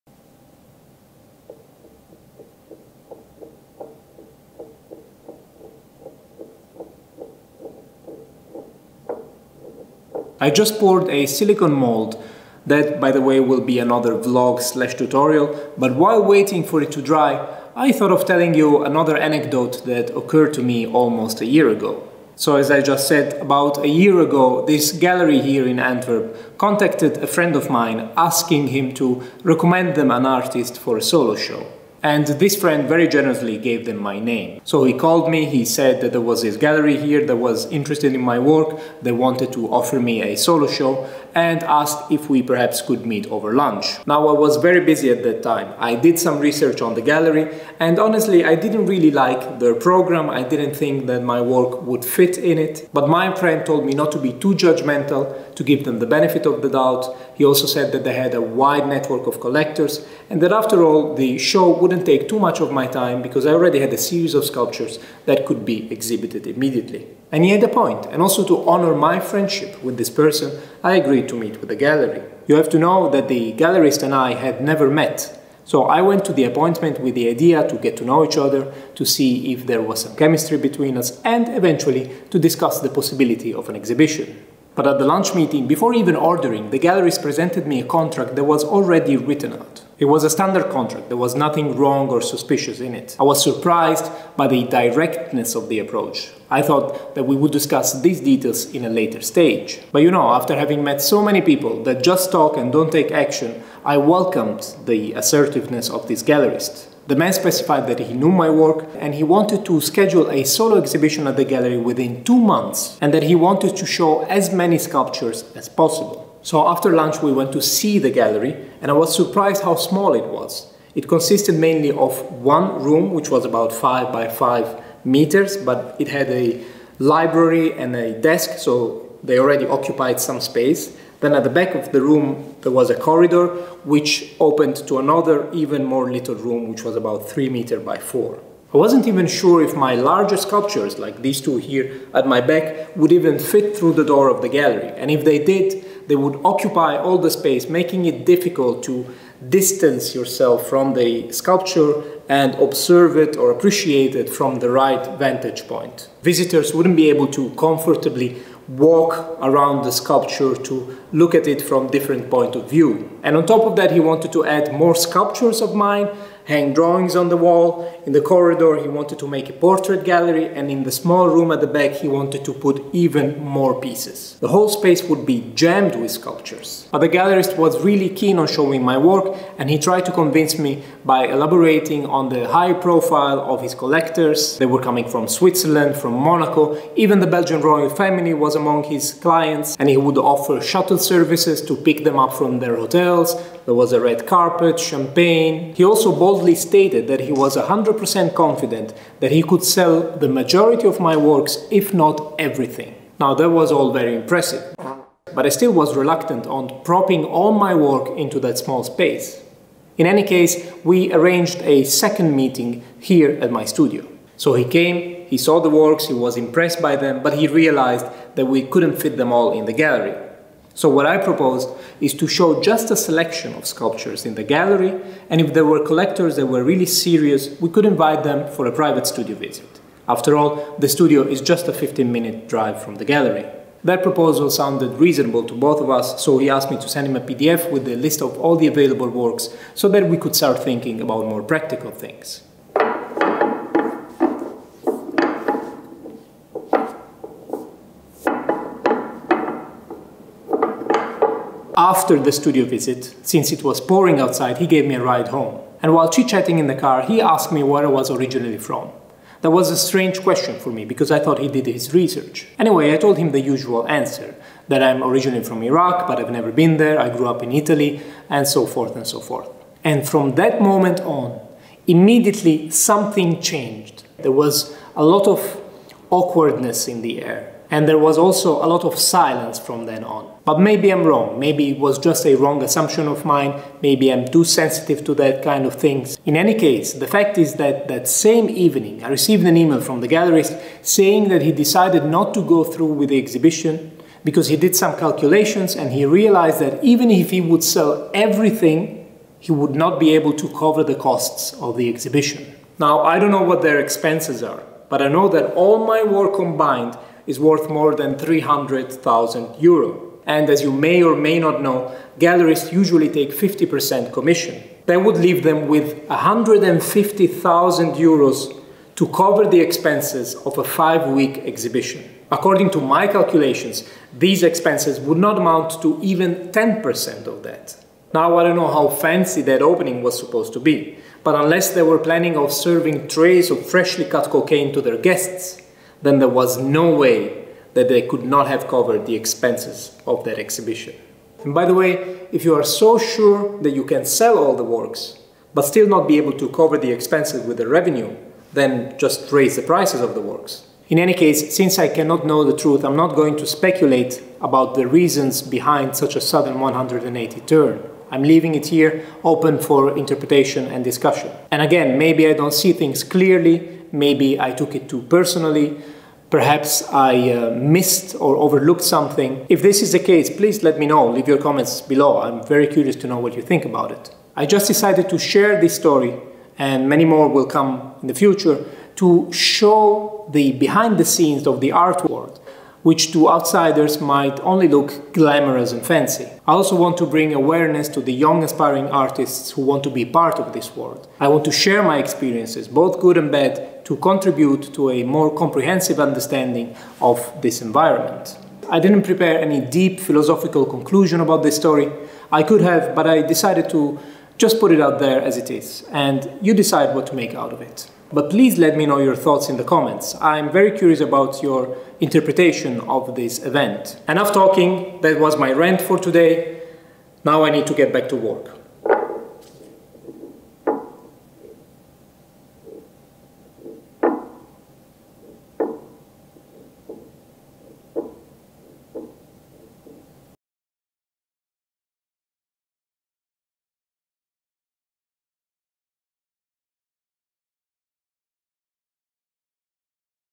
300K Was Not Enough!
A rant about contemporary art galleries, prejudice and maybe racism?